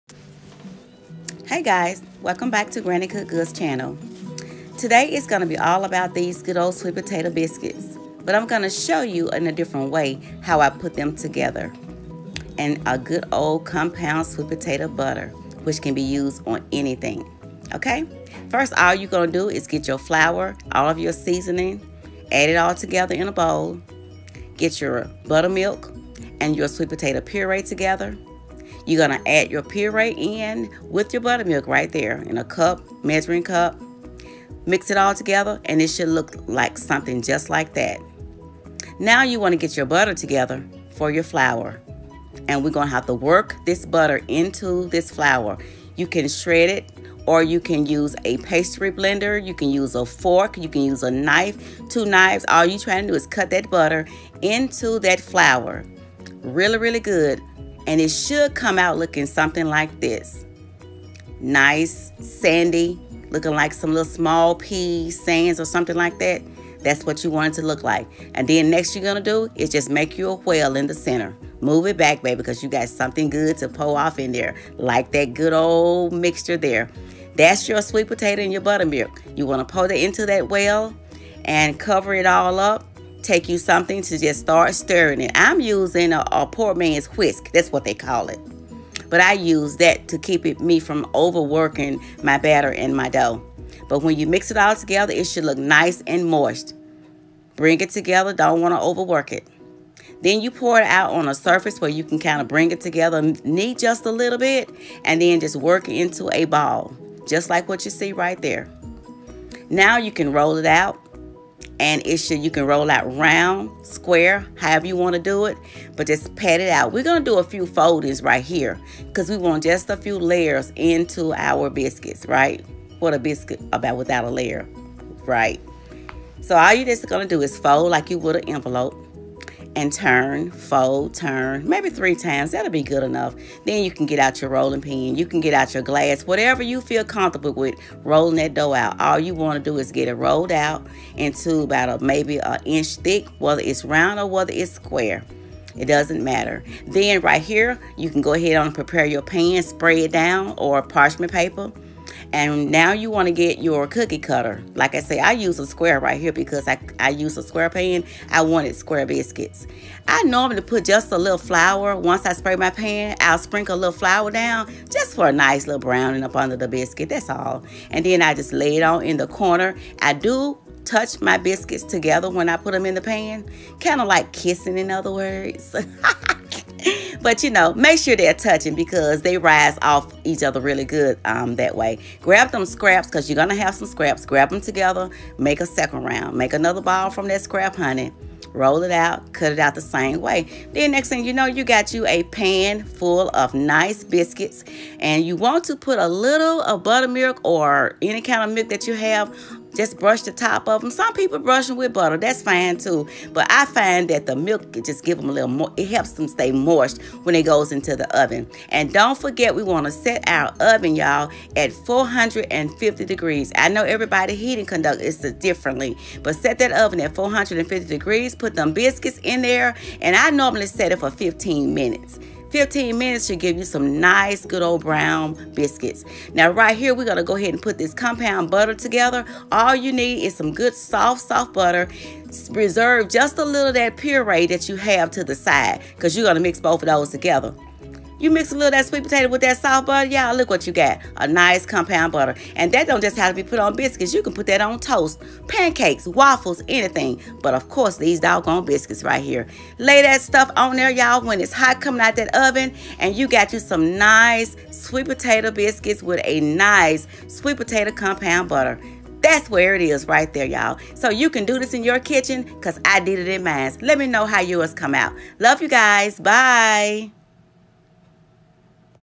SWEET POTATO BISCUITS | 🛑 STOP AND CHECK OUT THIS SHORT CLIP😋👌🏽
Heyyy Guy here is the recipe for these biscuits:
21/4 cup of A P Flour
3/4 tsp Salt
2 Tbs of Baking Powder
2 Tbs Sugar
1cup Buttermilk
1 Stick of butter
1/2 - 3/4 cup Sweet Potato Puree( I baked my sweet potatoes in the oven for 1 hour because they were small potatoes), you also can boil them, either way when done, peel snd smash ( add 1/4 nutmeg, cinnamon & ginger and 1tsp vanilla extract) stir to combine it should be whipped to a puree . Just follow my pictures video step by step to make these good old fashioned biscuits. I love and appreciate all if you.!!! 😘👋🏽